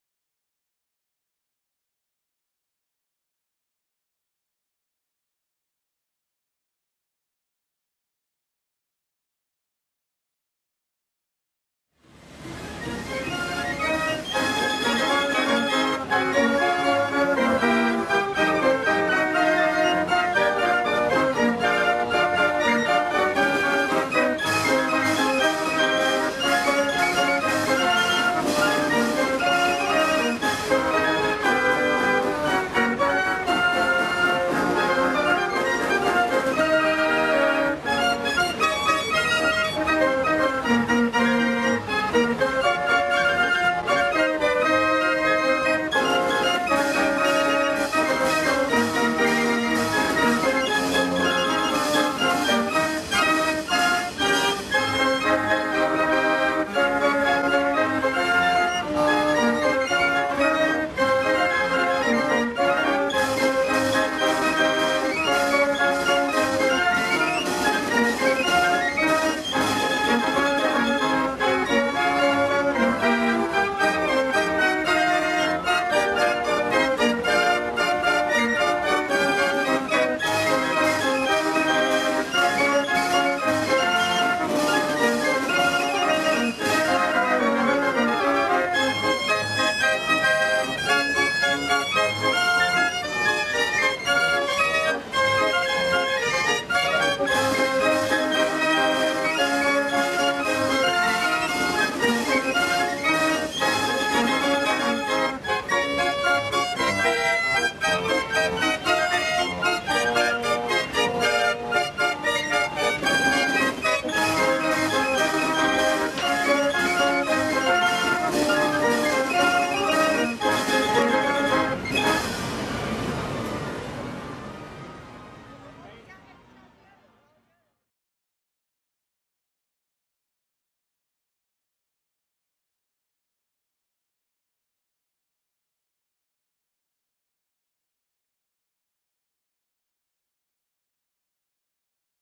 Lady Hamilton at Widnes 2005
The "Lady Hamilton" Marenghi organ at the Victoria Park rally in Widnes, Cheshire, September 2005. It was owned by Mike Priestner.

Sadly, the Bedford TK vehicle containing the organ was stolen and has never been recovered.